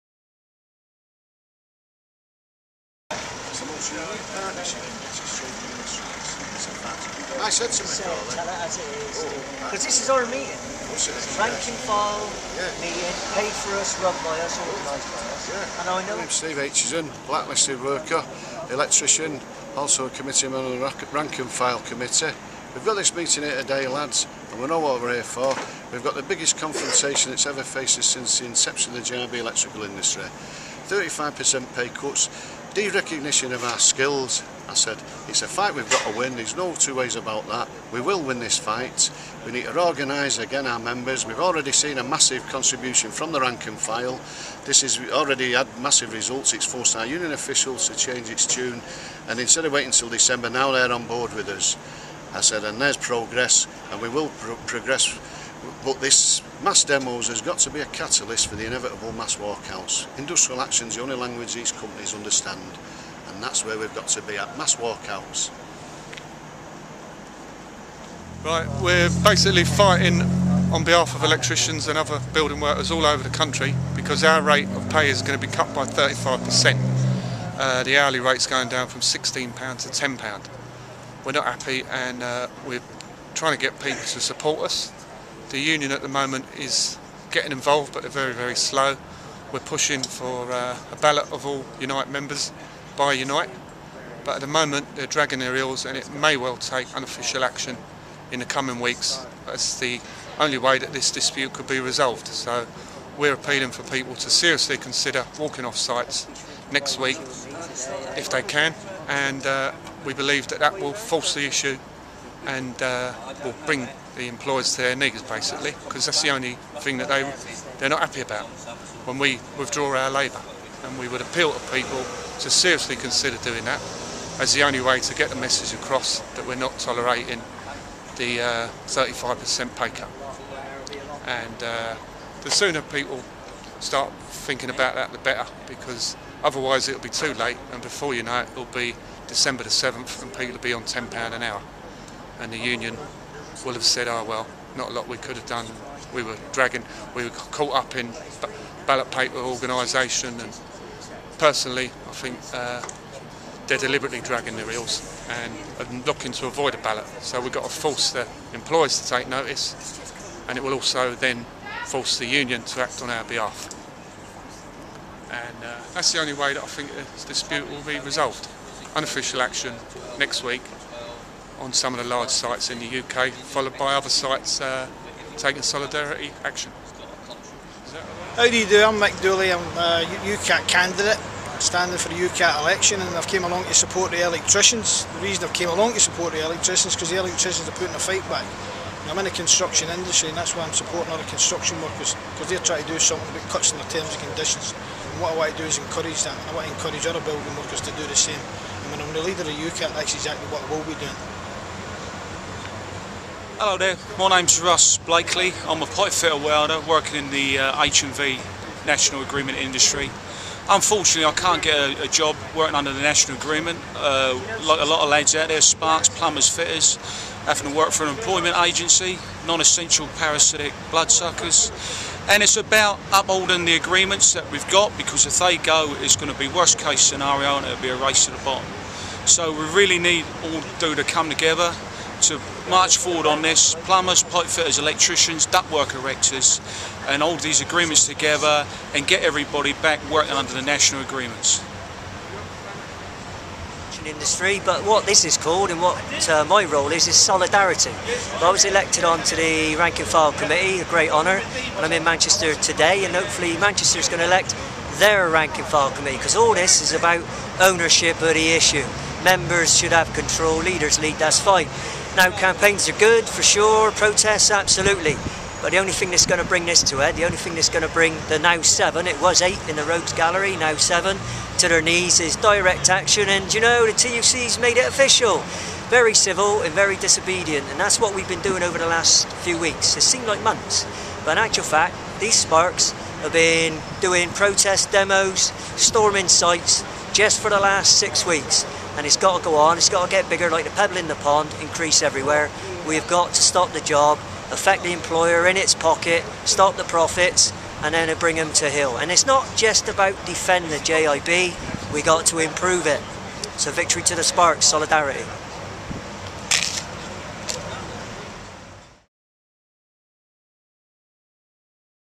Sparks of Resistance !.
Short reports from key players in current dispute in Construction industry where employers are attempting to cut wages and other conditions in JIB agreements. These interviews took place before the launch meeting of North West Committe in  Manchester